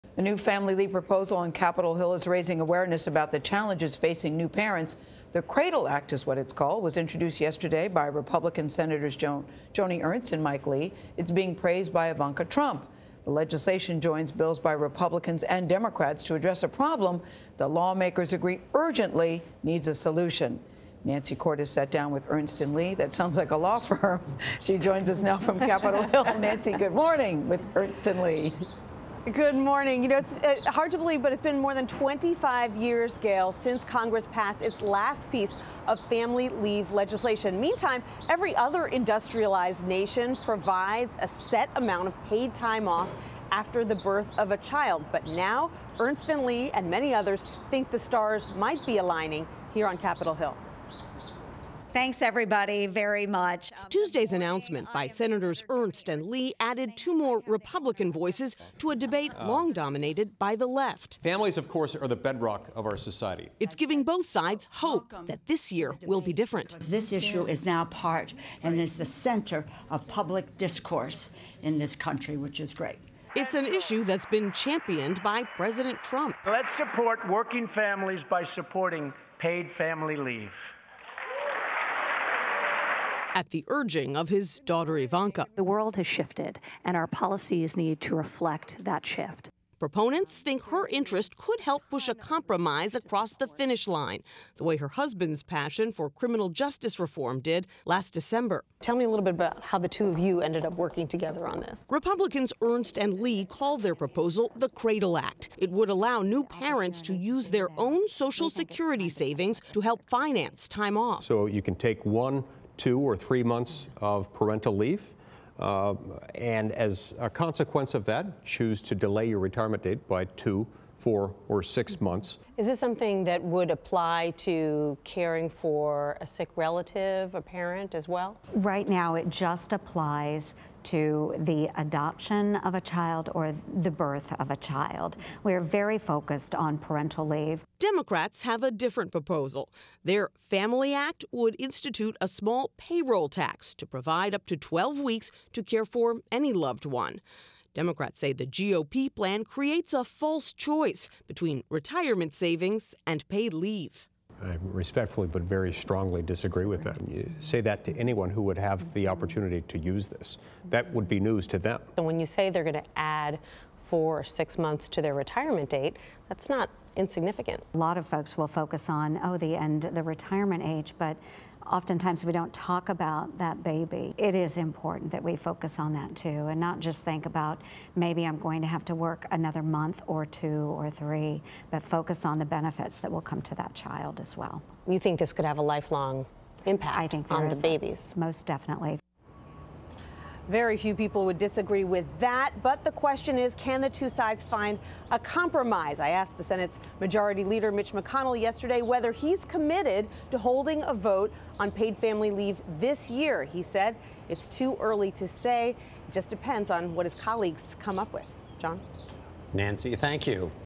How the Republican family leave proposal works
A new family leave proposal on Capitol Hill is raising awareness about the challenges facing new parents. The Cradle Act was introduced Tuesday by Republican Senators Joni Ernst and Mike Lee. Nancy Cordes sat down with Ernst and Lee to discuss the proposal.